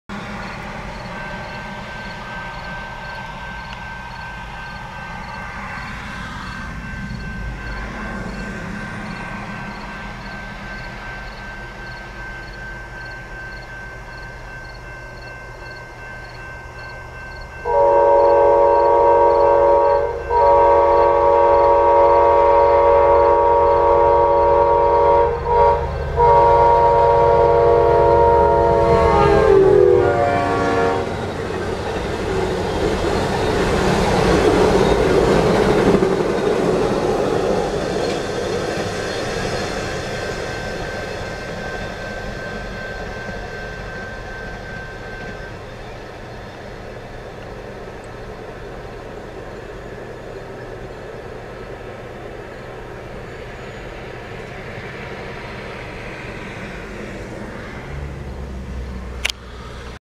AMTK 170 West AMTK Filmed In Welch Township MN (8-20-2022)
AMTK 170 (GE P42DC)
AMTK 305 (Siemens ALC-42)
West Passenger Train Chicago, IL-Seattle, WA/44 Axles-890 Feet/Main Track/DOT#391217P-M.P. 379.99
CP River Sub/10:00pm